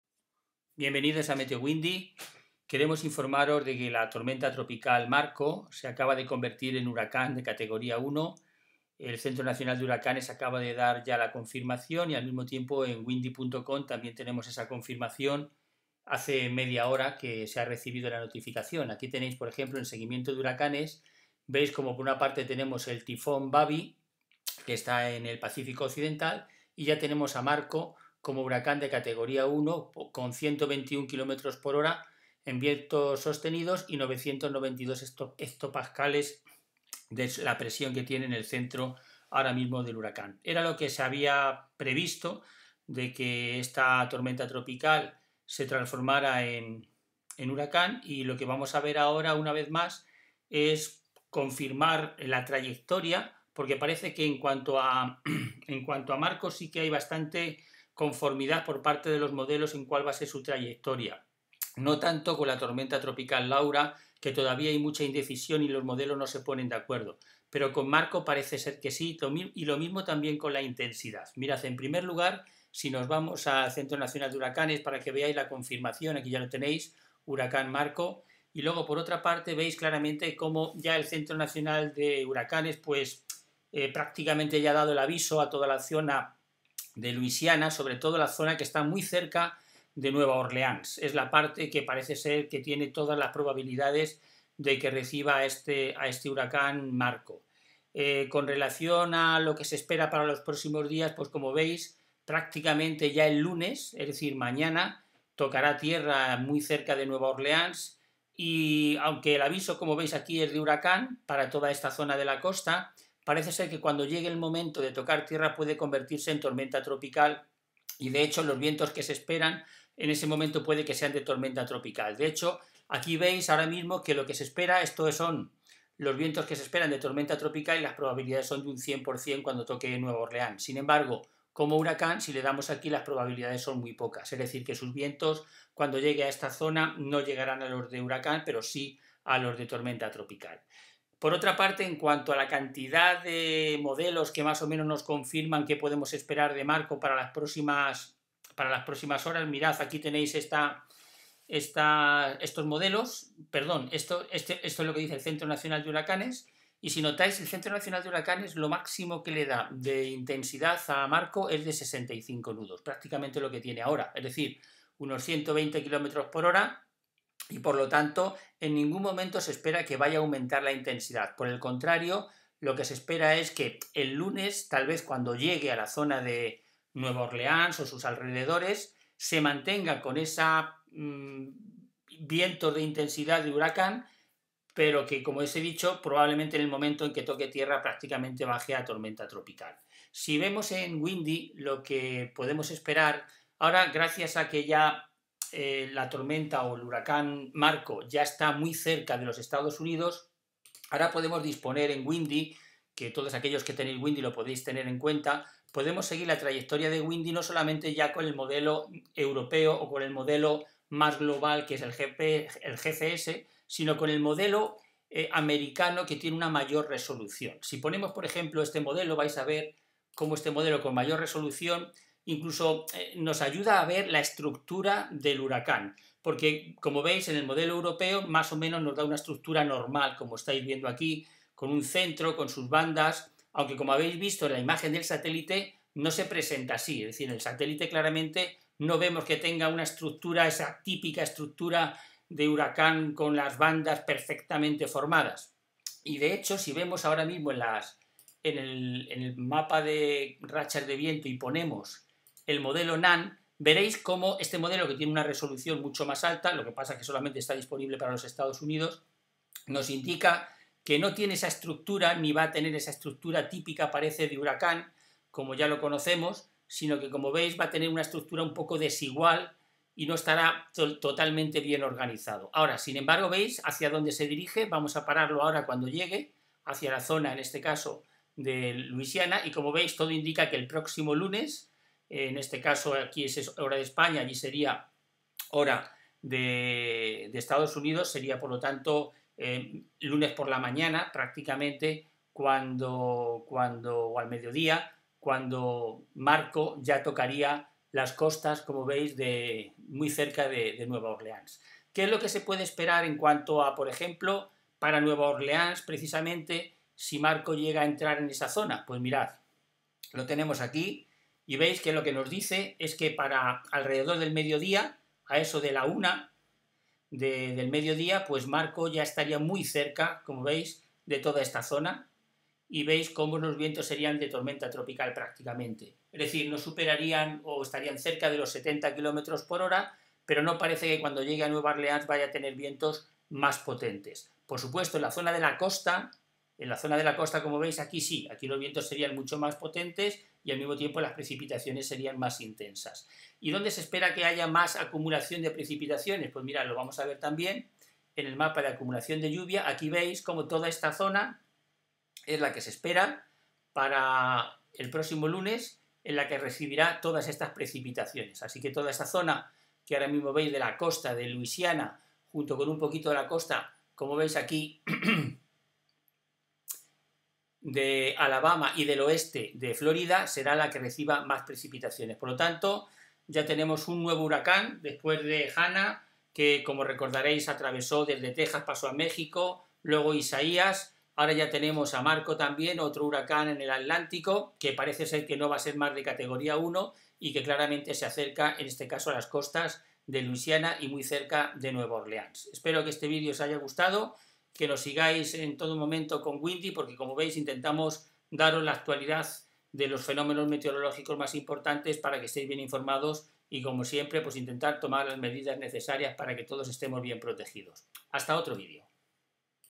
El HURACÁN MARCO se dirige a Luisiana. 23-8-2020
El Huracán Marco se dirige a Luisiana. El Centro Nacional de Huracanes le ha dado la categoría de Huracán 1 por tener vientos sostenidos de 121 km/h y se va acercando a las costas de Estados Unidos, en especial al estado de Luisiana, y los modelos indican que puede afectar a la zona de Nueva Orleans y todos sus alrededores.

El Huracán Marco llegará como huracán de categoría 1 y parece que bajará a Tormenta Tropical en cuanto toca la costa de Estados Unidos.